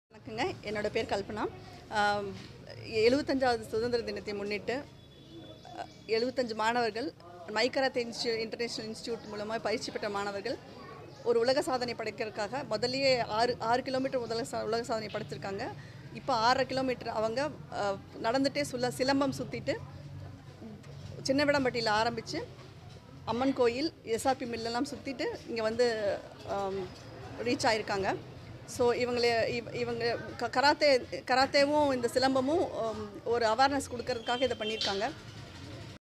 சிலம்பம் சுற்றியபடி நடை பயணம் செய்து மை கராத்தே பயிற்சி மைய மாணவ,மாணவிகள் நோபல் உலக சாதனை…
ஆறரை கிலோ மீட்டர் கையில் சிலம்பம் சுற்றியபடி நடை பயணம் செய்து மை கராத்தே பயிற்சி மைய மாணவ,மாணவிகள் நோபல் உலக சாதனை…

இந்துஸ்தான் ஸ்கௌட்ஸ் மற்றும் கைட்ஸ் அசோசியேஷன்,மை கராத்தே இண்டர்நேஷனல், கோவை மாவட்ட சிலம்பம் கமிட்டி ஆகியோர் இணைந்து கோவையில் 75 வது சுதந்திர தினத்தை வரவேற்கும் விதமக புதிய நோபல் உலக சாதனை நிகழ்ச்சியை நடத்தியுள்ளனர்.இதில் மை கராத்தே பயிற்சி மையத்தை சேர்ந்த மாணவ,மணவிகள் கையில் சிலம்பம் சுற்றியபடி ஆறரை கிலோ மீட்டர் நடைபயணம் மேற்கொண்டு இந்த சாதனையை செய்துள்ளனர்..நோபல் உலக சாதனை புத்தகத்தின் நிறுவனர் தியாகு தலைமையில் நடைபெற்ற இதன் துவக்க நிகழ்ச்சியில் நோபல் உலக சாதனை புத்தகத்தின் தீர்ப்பாளர்கள் சிவமுருகன்,அரவிந்த்,சிவலிங்கம், விது சங்கர் ஆகியோர் சாதனை நிகழ்வை ஒருங்கிணைத்தனர். சின்னவேடம்பட்டி பகுதியில் துவங்கிய நடைபயணம் பல்வேறு வீதிகள் வழியாக பிஸி பீ நர்சரி பள்ளியில் நிறைவு பெற்றது..தொடர்ந்து சாதனை புரிந்த மாணவ,மாணவிகளுக்கு பிசி பீ பள்ளியின் தாளாளர் கல்பனா முரளிதரன் பாராட்டுகளை தெரிவித்து சான்றிதழ் மற்றும் பரிசுகளை வழங்கினார்..75 வது சுதந்திர தினத்தை வரவேற்கும் விதமாக செய்த இந்த சாதனை நோபல் உலக சாதனை புத்தகத்தில் இடம் பிடித்தது..சாதனை புரிந்த மாணவ,மாணவிகளுக்கு பெற்றோர்கள்,பொதுமக்கள் மற்றும் இணை ஒருங்கிணைப்பாளர்கள் சரவணன்,நந்தகுமார், பிரபாகரன் உள்ளிட்ட பலரும் பாராட்டுகளை தெரிவித்தனர்..